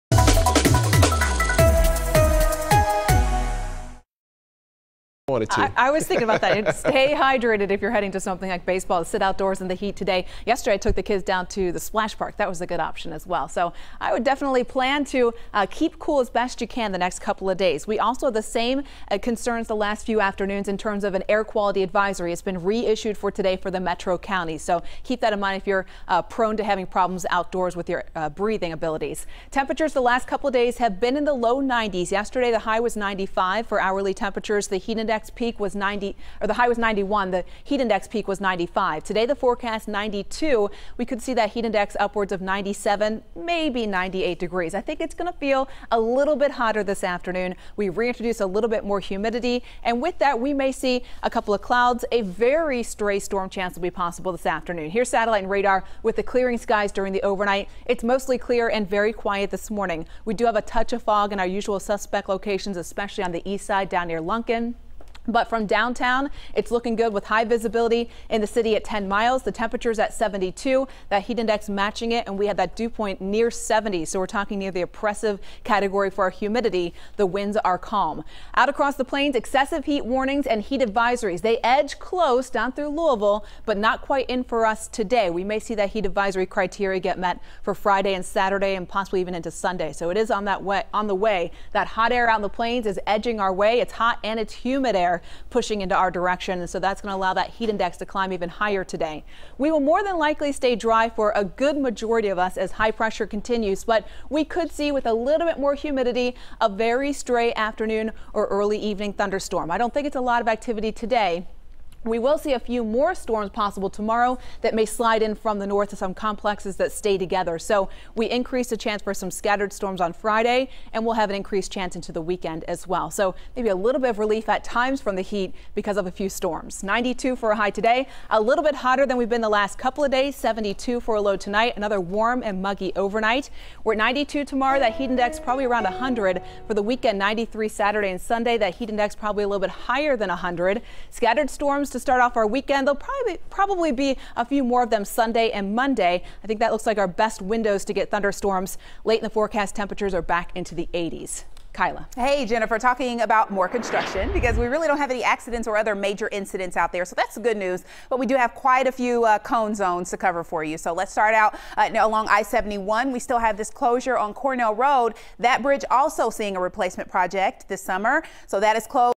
Hot and Humid
Temperatures climb back into the low 90s again today but the heat index will be above 95.